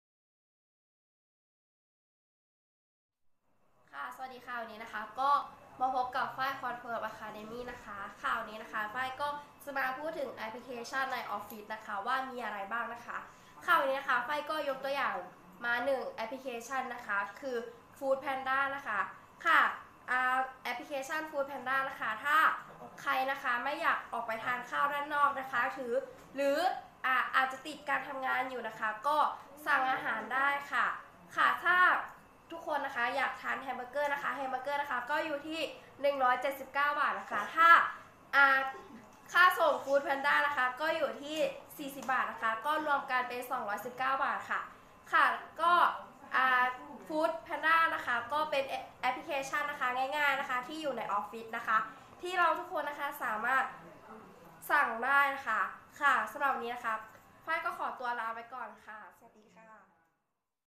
แอบน่าใช่ใน office Confirmedtour
แอพน่าใช้ใน office - food panda

แอบ food panda คือแอปพลิเคชัน 1 ที่ใช้ในความสะดวกสบายในการสั่งอาหารหรือส่งอาหารการที่สั่งอาหารที่ต่างๆแล้วจ้าง food Panda ก็จะมีค่าส่งอาหารราคา 40 บาทตามระยะทางถ้าไกลก็จะเพิ่มเงินการสั่งกับ food panda มีความสะดวกสบายและรวดเร็วและตรงต่อเวลาถ้าใครที่ชอบความสะดวกสบายและไม่ชอบการที่ออกไปทานอาหารด้านนอกก็สั่งอาหารโดยที่ใช้  food panda มาส่งได้